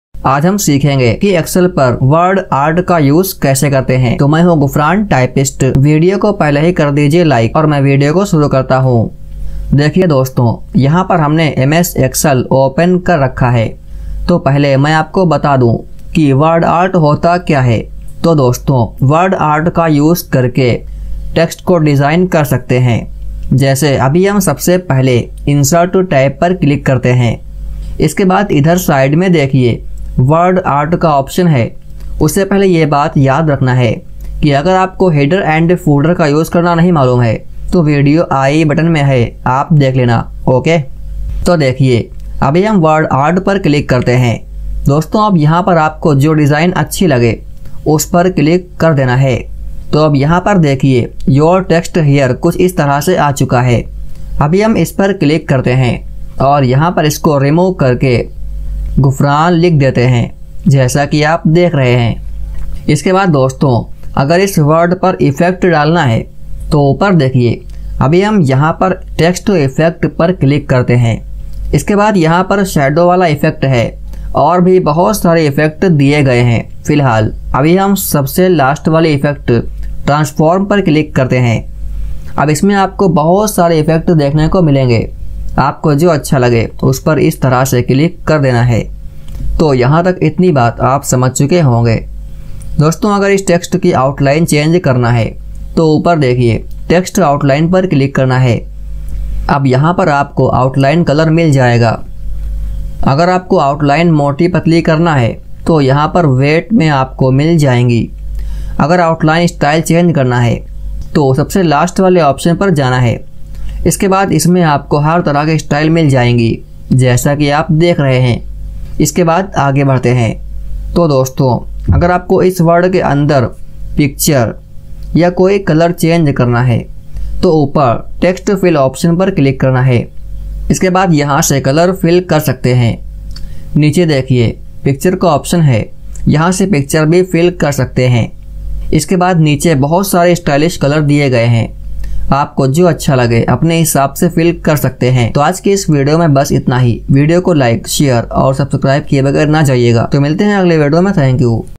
How To Use Word Art in Excel? | Excel पर Word Art Option के Master बनें दो मिनट में
About this video: Friends, is video me maine aapko ms excel per wordart ka use krna bataya hai, Umeed hai kiye video aapko zaroor pasand ayegi...

2) how do i insert wordart in excel?
4) how do you do wordart in excel?